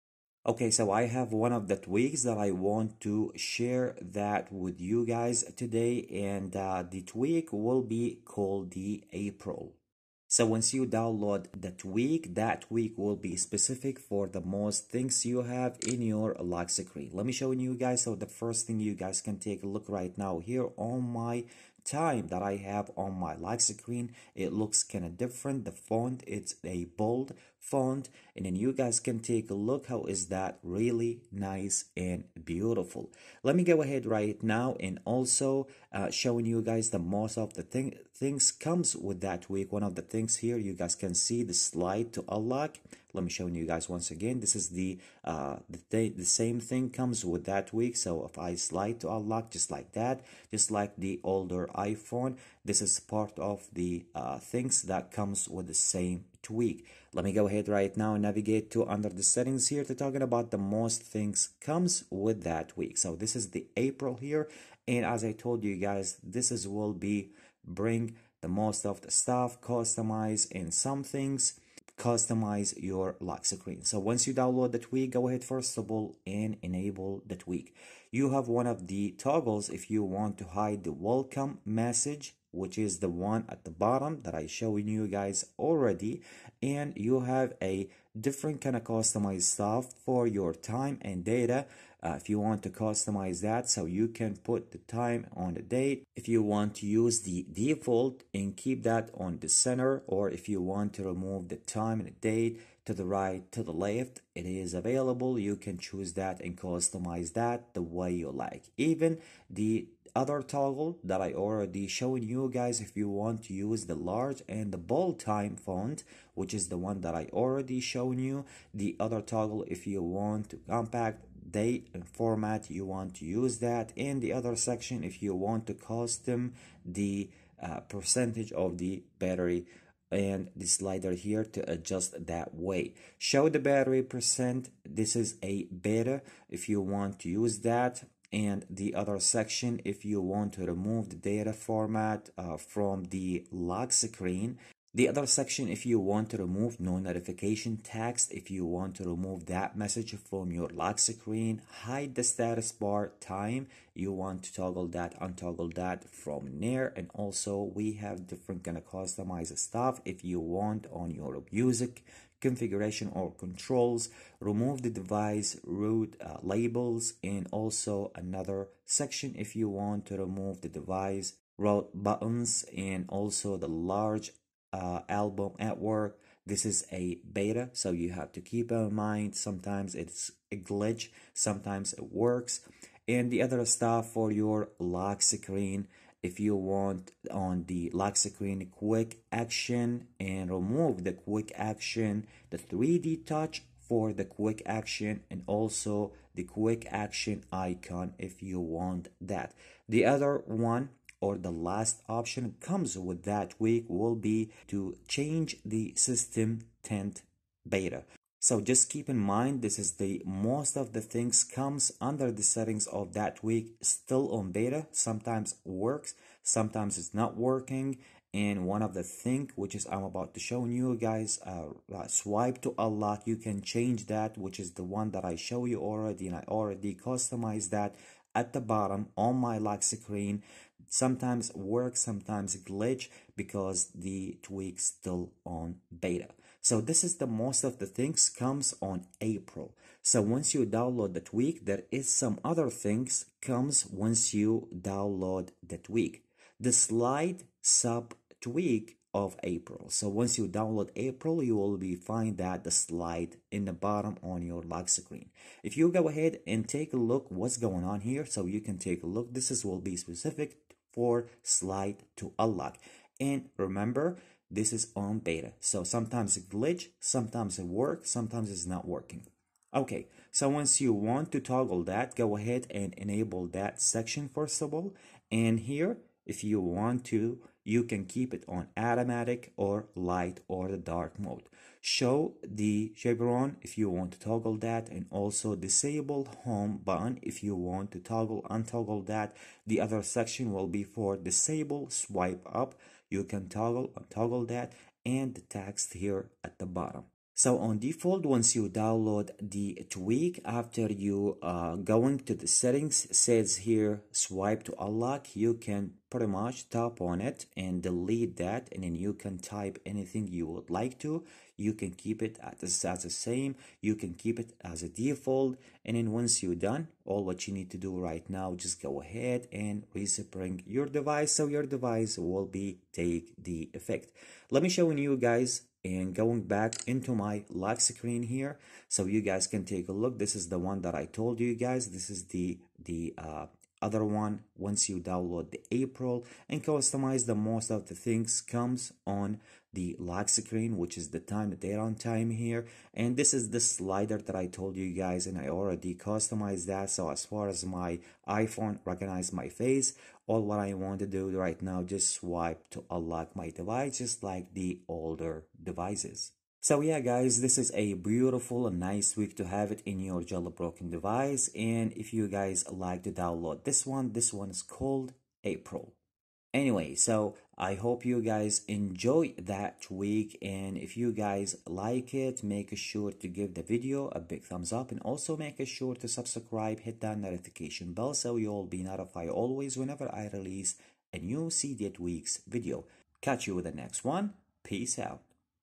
Most Download Tweak New A12 - A13 Jailbreak Tweaks for iOS 13 / 13.4.1! (Unc0ver + Checkra1n)
iOS13.4 Perfect Jailbroken iPhone: Top 13 BEST iOS 13 Jailbreak Tweaks for Cydia. Customize your iPhone on iOS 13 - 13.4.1 and get NEW features on the Unc0ver / Checkra1n Jailbreak

Theme :  Lotus